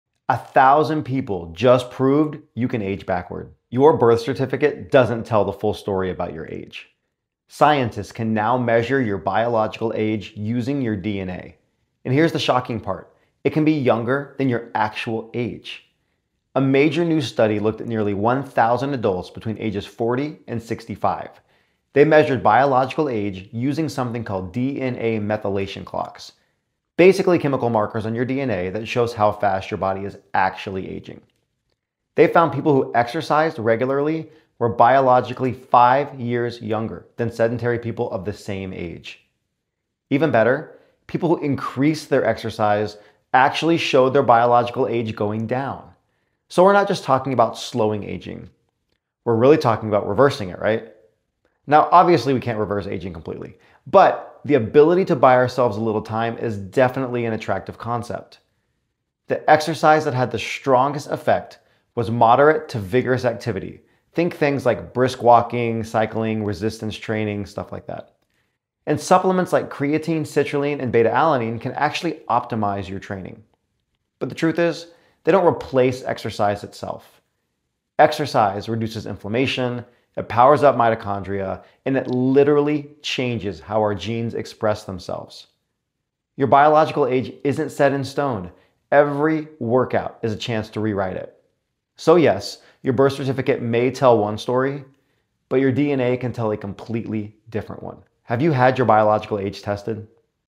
What if aging was optional? 🤔
New research shows regular exercise can make you biologically 5 years younger than your actual age.